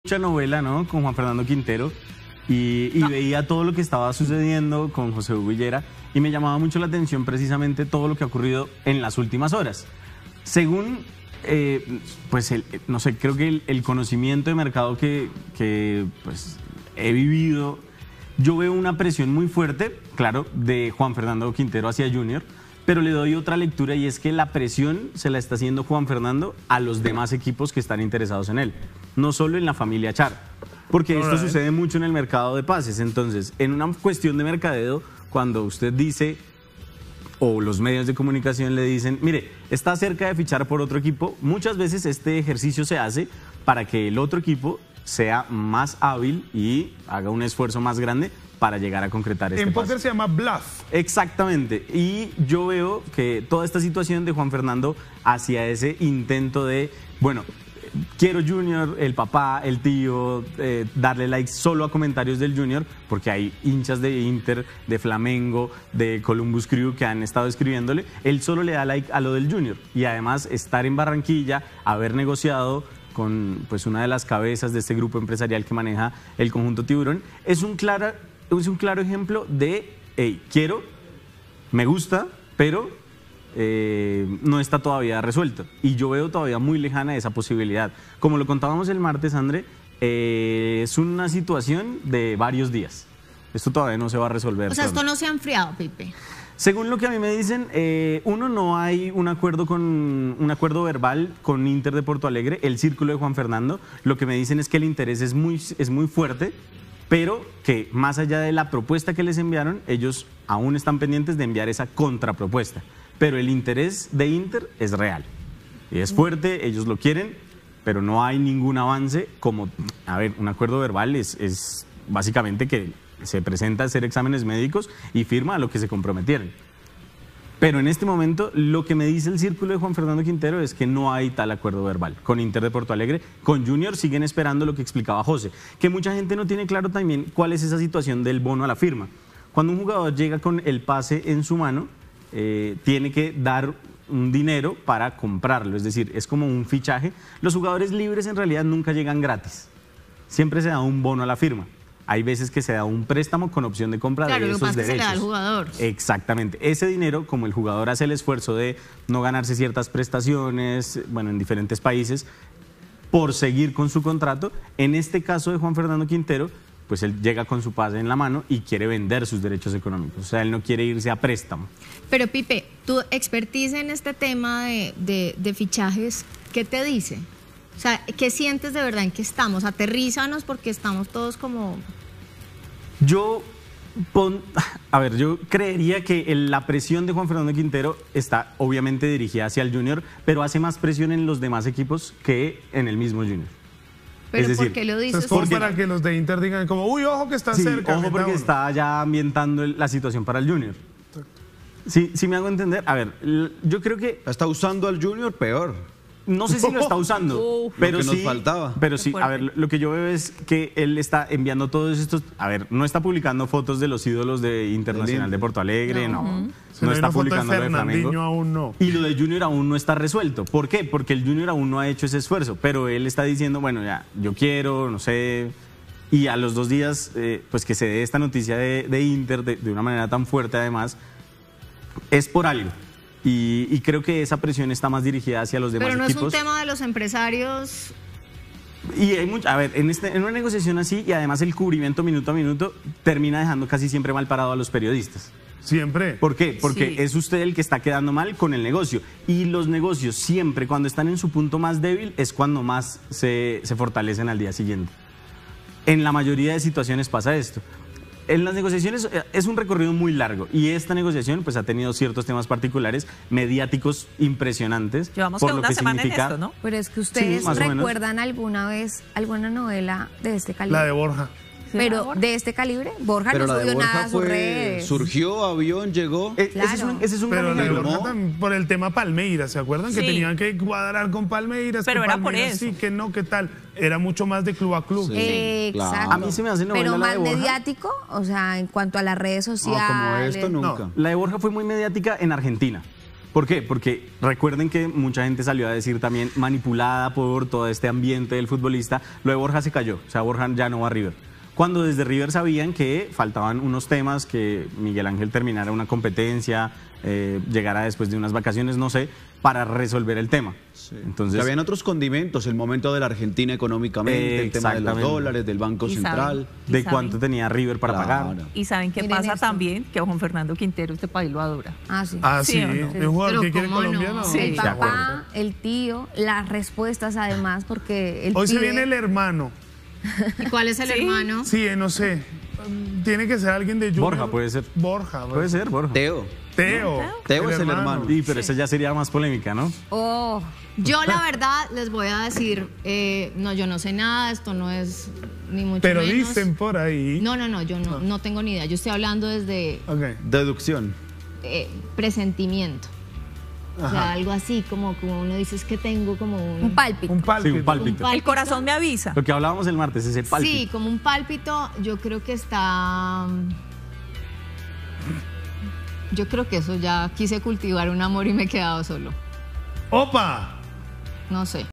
¡Rumbo a Brasil! Así van las negociaciones de Inter de Porto Alegre con Juan Fernando Quintero
El cuadro brasileño ha mostrado gran interés en el colombiano.